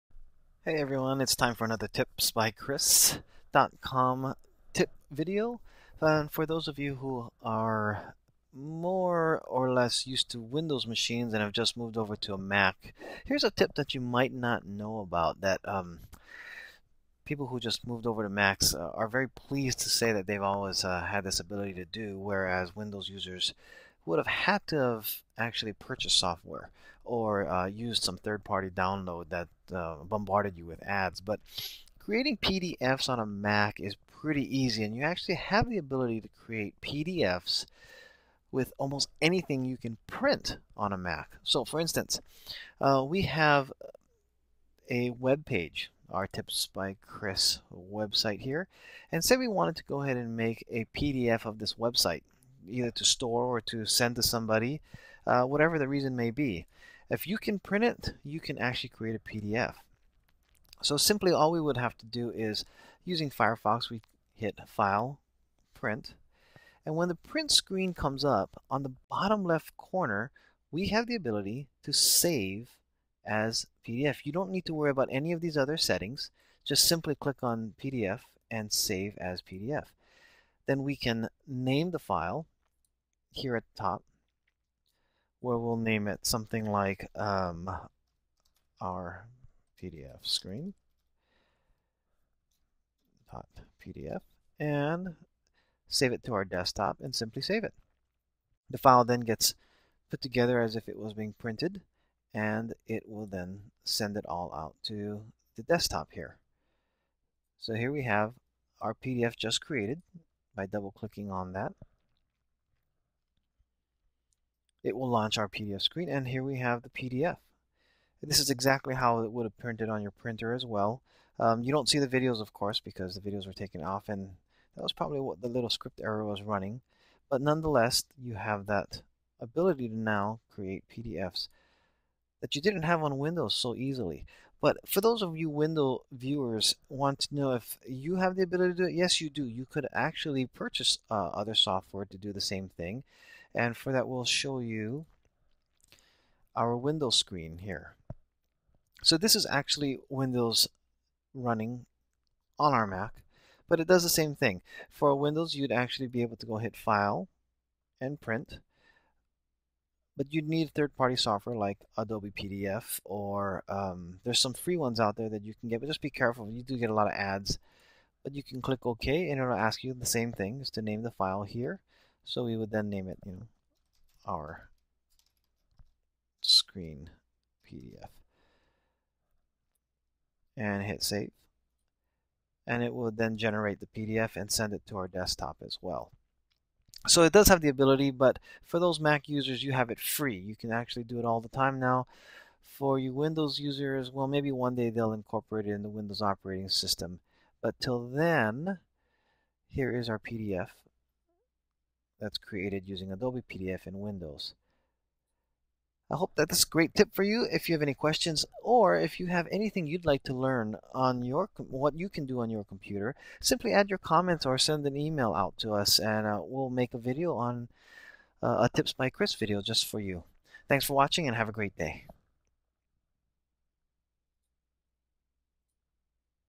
How To Create PDF's Of Almost Anything With a Mac or Windows PC
- One of the coolest things "new" MAC users will find over a Windows computer is the ability to easily create PDF (aka Portable Document Format) documents of almost anything you can print.
If you can get to a print menu, you can create a PDF.
For Windows users, there are a few free PDF creators downloadable on the internet, but be aware of the fact that you will often deal with closing advertisements anytime you use the downloaded software.
I have adobe acrobat (which I accidentally called "adobe PDF" in this video) on my Windows 7 computer to do more than just print to PDFs, but also create forms and work inside the actual forms. It wasn't cheap, and may not be cost effective if you don't have to do more advanced work (like securing a PDF with a password) etc.